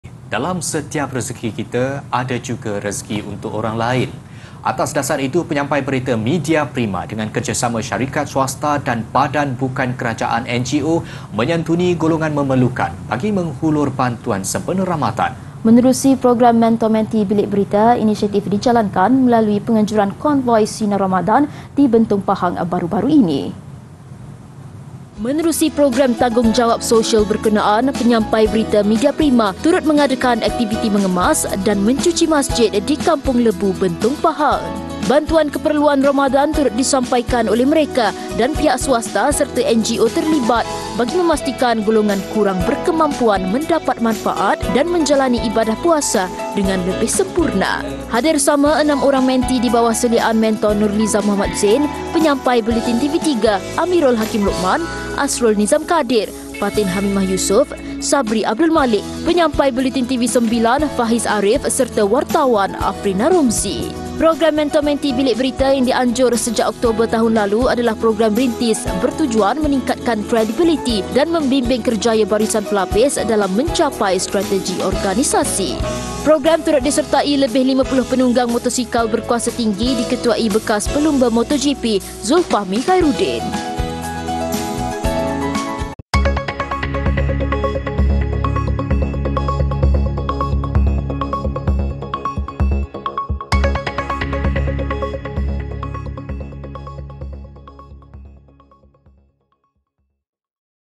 Mentor-Mentee Bilik Berita MPB Konvoi Santuni Asnaf
Di dalam setiap rezeki kita, ada juga rezeki untuk orang lain.

Atas dasar itu, penyampai berita Media Prima dengan kerjasama syarikat swasta dan badan bukan kerajaan, NGO menyantuni golongan memerlukan bagi menghulur bantuan sempena Ramadan.